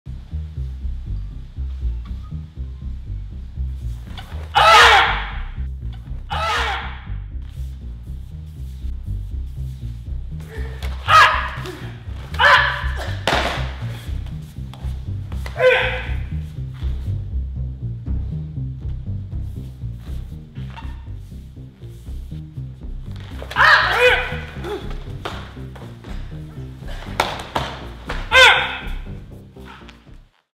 Breathtaking Display of the Martial Art of Shorinji Kempo
While Japanese Shorinji Kempo was only founded in 1947, its roots go all the way back to ancient China and Shaolin Temple. An important aspect of training is paired practice where opponents go through attack and defense drills to sharpen technique. It can be very dynamic. We shot this demonstration at the headquarters or Hombu Dojo of Shorinji Kempo in Todatsu on the island of Shikoku, Japan.